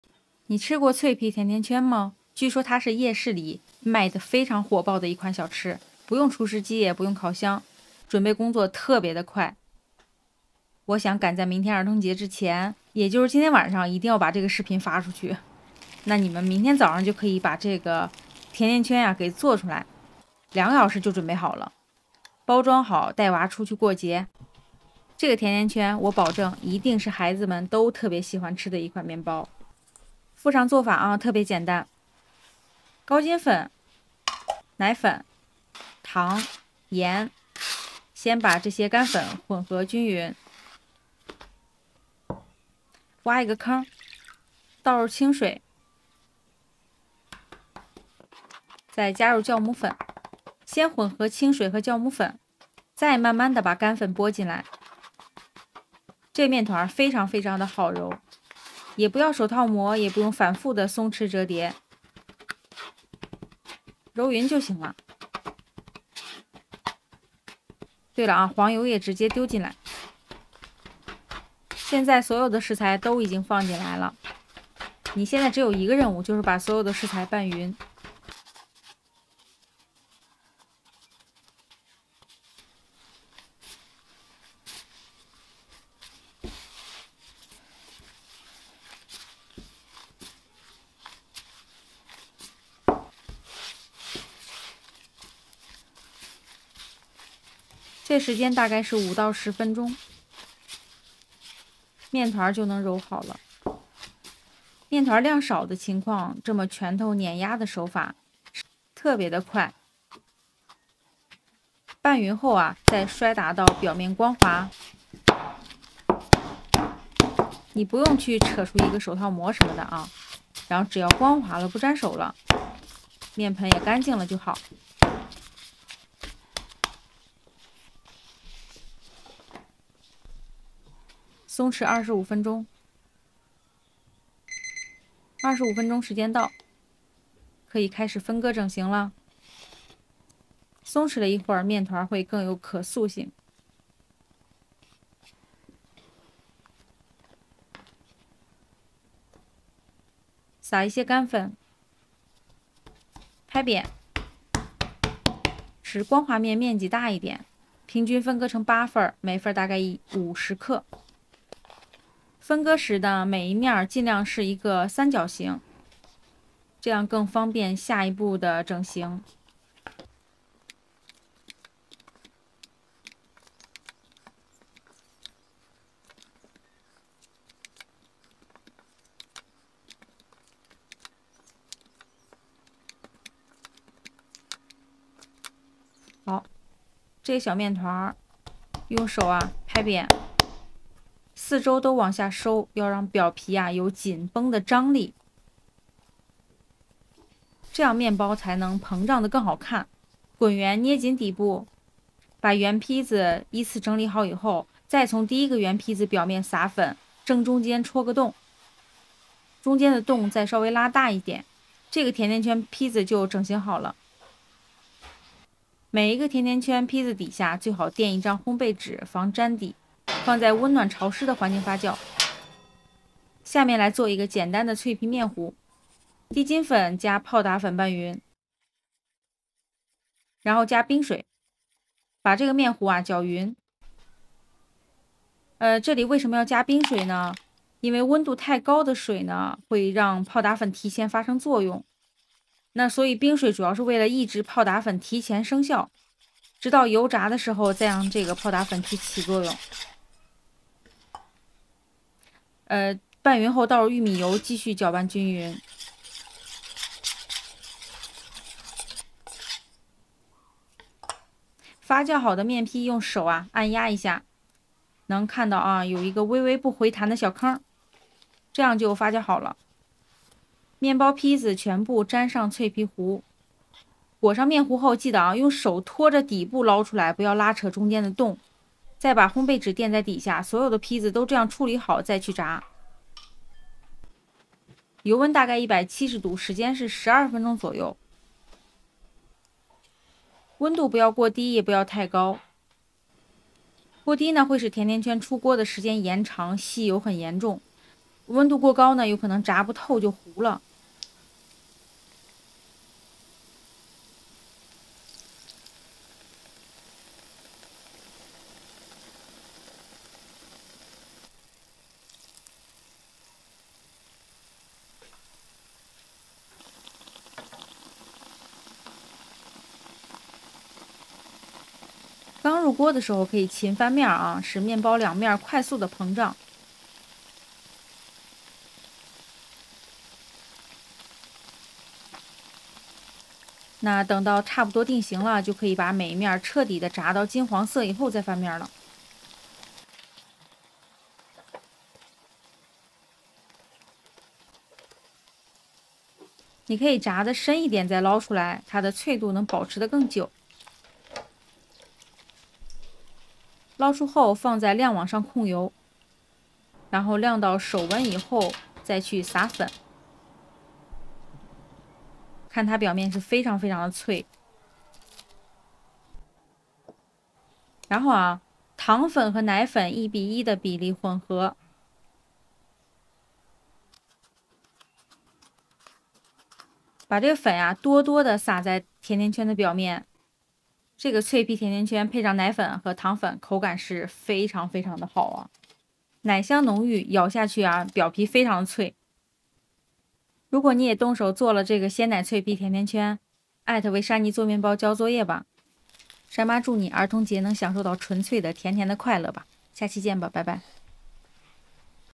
解锁夜市小吃鲜奶脆皮甜甜圈，不用厨师机不用烤箱，原来这么简单【为珊妮做面包】
哈喽~ 大家好，我是为珊妮做面包的珊妈

「附配方」
面团部分（8个甜甜圈）：高筋粉200克、奶粉20克、糖16克、盐3克、水125克、酵母粉3克、黄油10克、 脆皮糊部分、低筋粉60克、泡打粉1克、冰水80克、玉米油10克；
撒粉部分：奶粉25克、糖粉25克。

我爱面包🍞，也爱烘焙🍰
更爱做给珊妮尽尝美味~

希望同样爱做面包的你能来和我交流分享